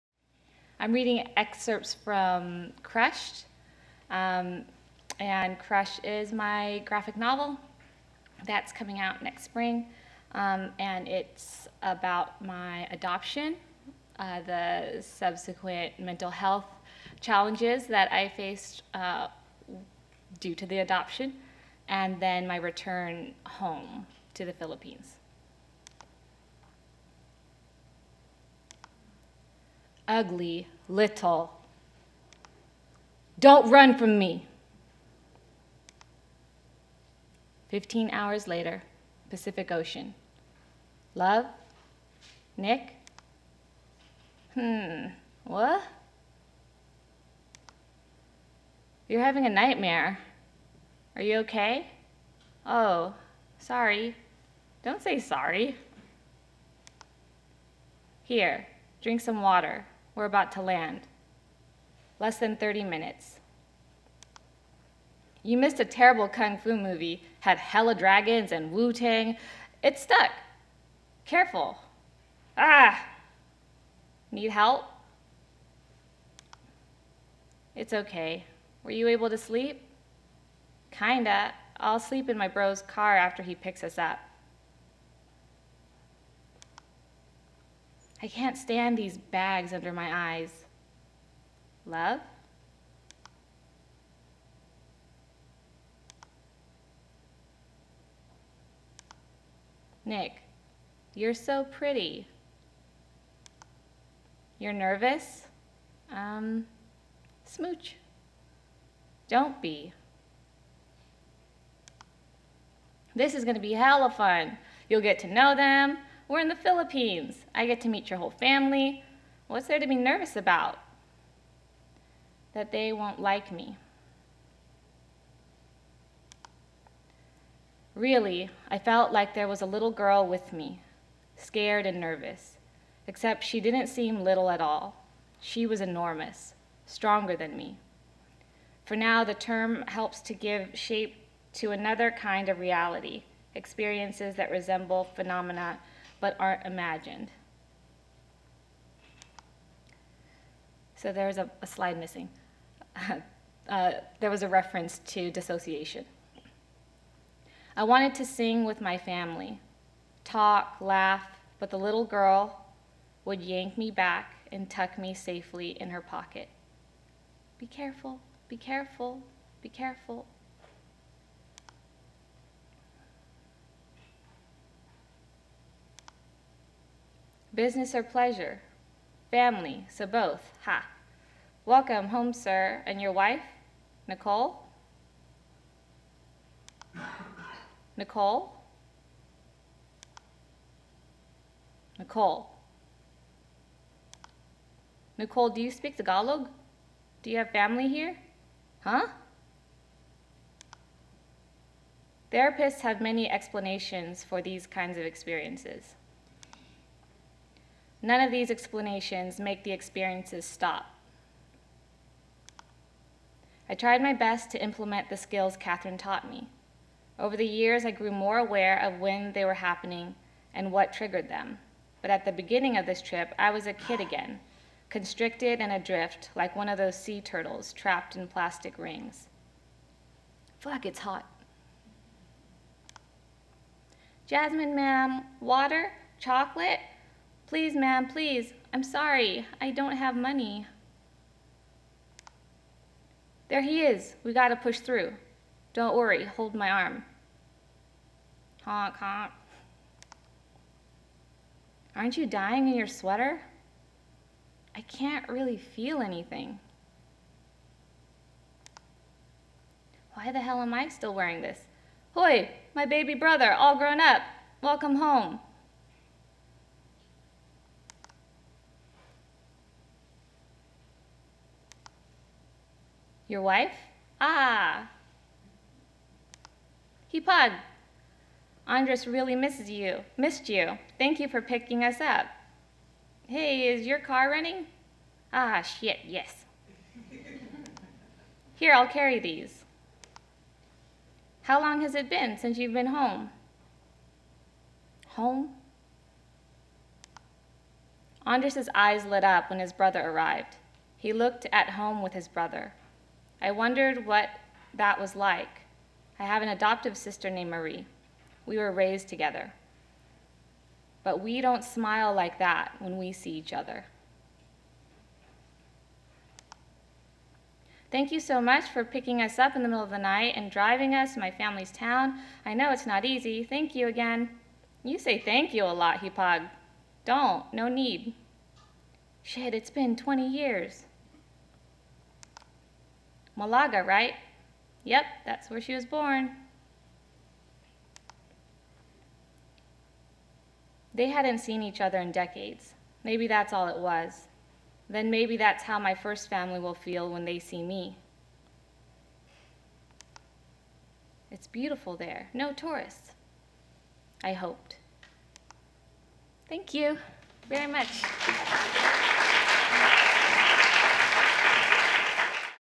Trinidad Escobar reads for SF Zine Fest at the San Francisco Public Library
Trinidad Escobar narrates her work at the Thinking Captions event, part of the 2017 San Francisco Zine Fest.